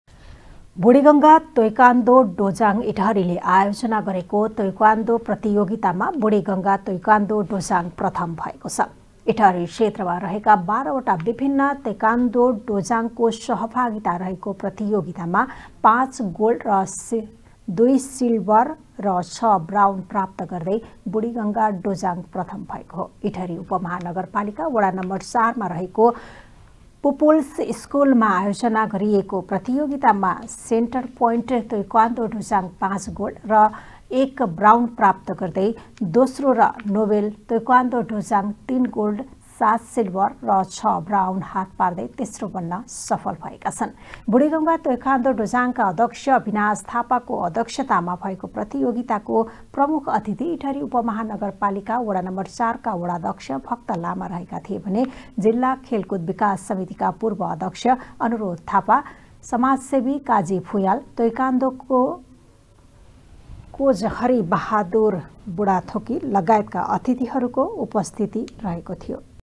बुढीगंगा तेक्वाण्दो डोजाङ इटहरीले आयोजनागरेको तेक्वादो प्रतियोगितामा बुढीगंगा तेक्वाण्दो डोजाङ प्रथम
बुढीगंगा तेक्वाण्दो डोजाङ इटहरीले आयोजना गरेको तेक्वादो प्रतियोगितामा  बुढी गंगा तेक्वाण्दो डोजाङ प्रथम भएको छ ।